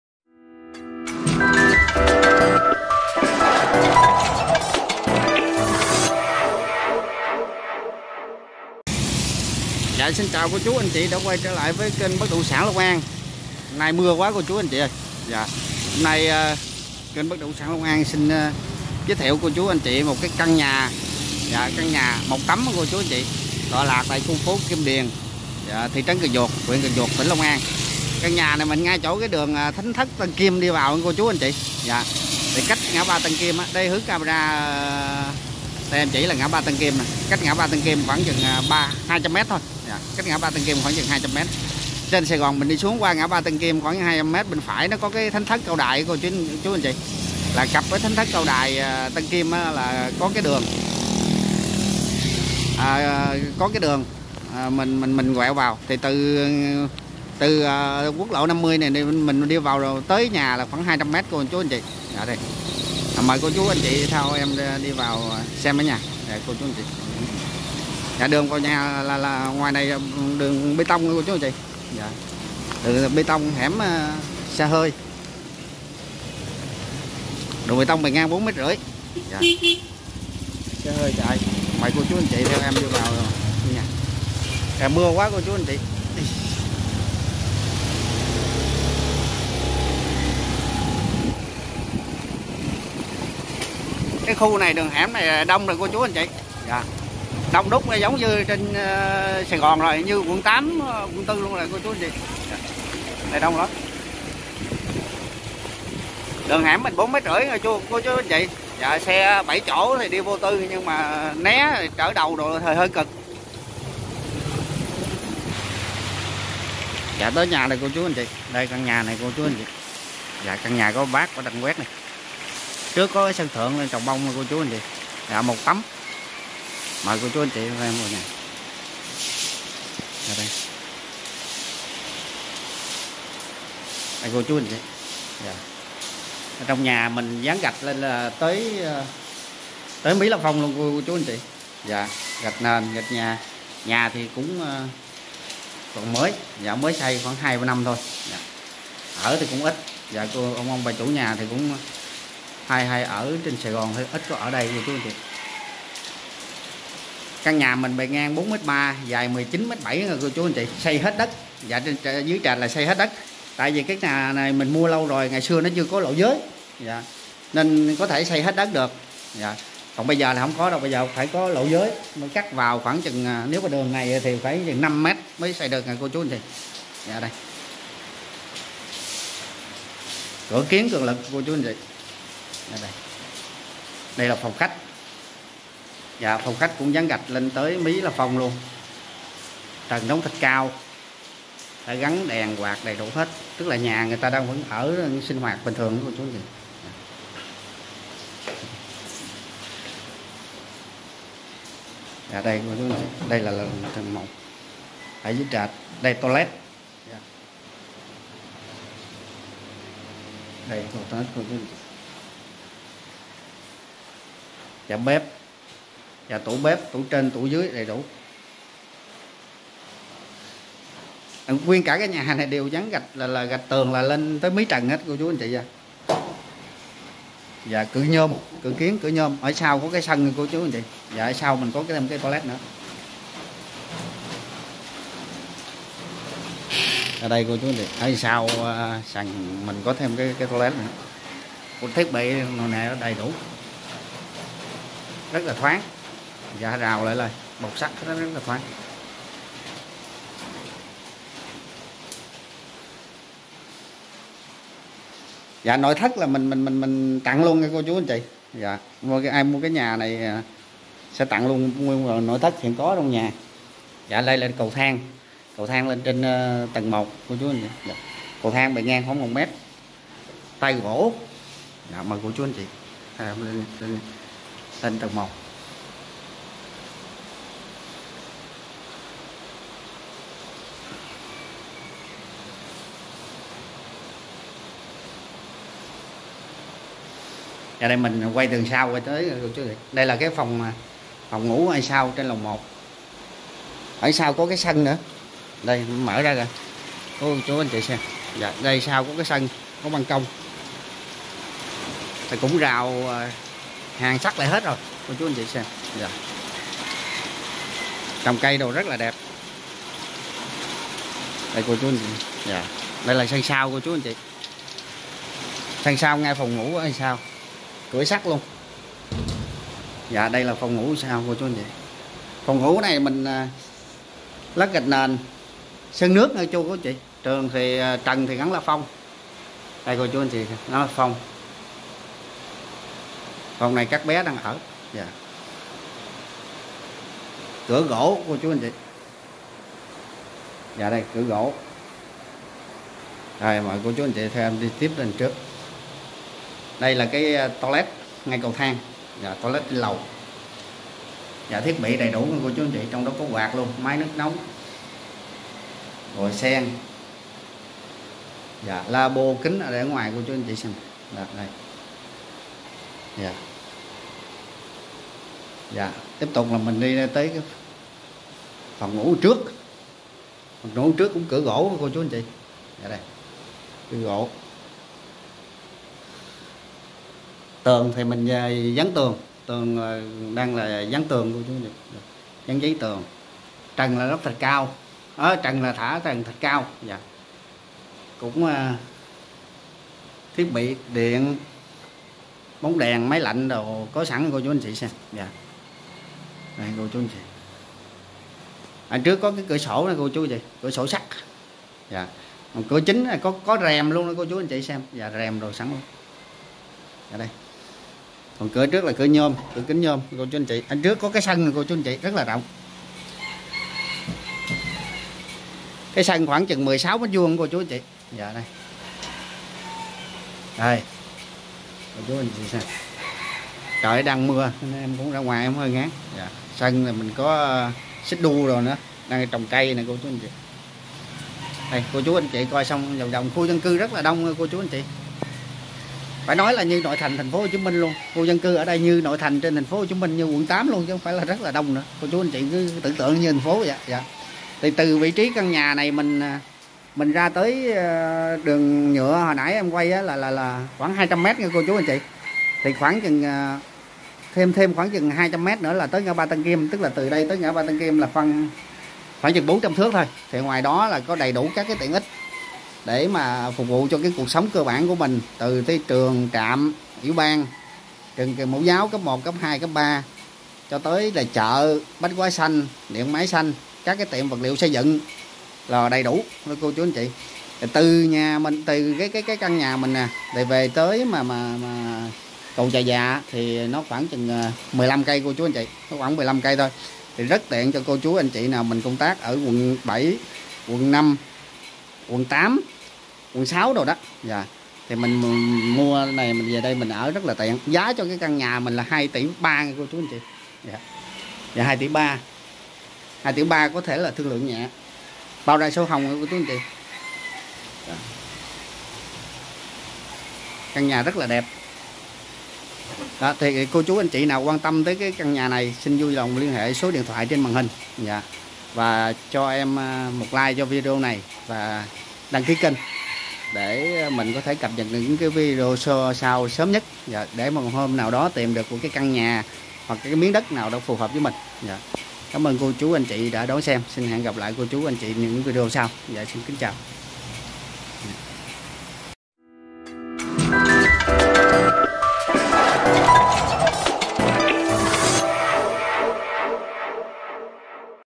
(Ms 297❤Đã Bán) Chuyển Về Thành Phố Nên Bán Căn Nhà Tâm Huyết Cần Giuộc, Long An | Bđs Long An
GIA ĐÌNH CHUYỂN VỀ THÀNH PHỐ NÊN BÁN CĂN NHÀ TÂM HUYẾT TÂN KIM, CẦN GIUỘC, LONG AN

Gia đình chuyển về thành phố sinh sống nên  đành bán căn nhà phố tâm huyết, hẻm thánh thất Tân Kim, QL50 khu phố Kim Điền, thị trấn Cần Giuộc, tỉnh Long An.
Khu vực nhiều tiện ích: Ủy ban, trường học, cây xăng, điện máy xanh...
Gần khu công nghiệp Tân Kim mở rộng.
Giáp ranh Bình Chánh Tp HCM.
Tiện để ở, mở văn phòng, mua bán, kinh doanh...
DT: 4.3 × 19.
Giá: 2.3Tỷ. SHR. BST.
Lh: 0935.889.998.

BĐS LONG AN

Chuyên:

Mua bán bất động sản
Dịch vụ bất động sản
Quảng cáo bất động sản
Tư vấn bất động sản